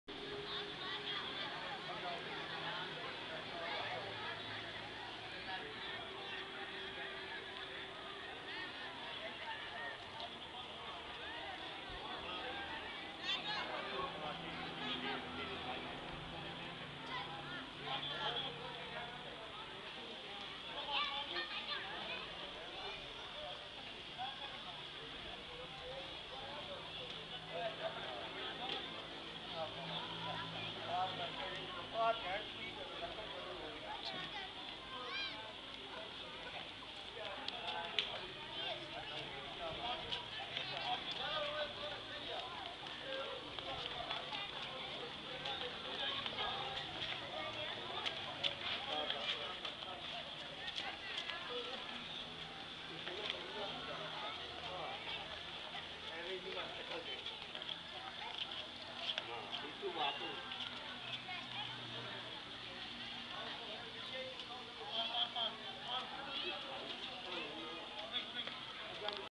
Mankiala Phattak
Mankiala Phattak -  Minute later TRAIN arrival !